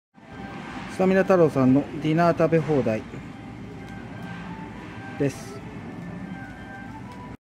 お食事処：すたみな太郎 宇佐店⑳ ～小林消防設備 福岡県豊前市 全類消防設備士 第二種電気工事士 経営学修士～
動画は、大分県宇佐市にある すたみな太郎 宇佐店さんのディナー食べ放題（90分）です。
ディナー食べ放題（90分）は、大人税抜き＠￥2,100・子供（小学生）税抜き＠￥1,200・幼児（3歳以上）税抜き＠￥400です。
お肉・お寿司・お惣菜・デザート・アイスなどが食べ放題です。